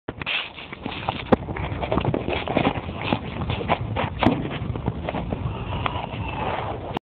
Rudolph The Red Nosed Reindeer, Ash End House Childrens Farm
Rudolph The Red Nosed Reindeer sniffing the camera at Ash End House Childrens Farm, Middleton, Tamworth. One Month Before Christmas 2007.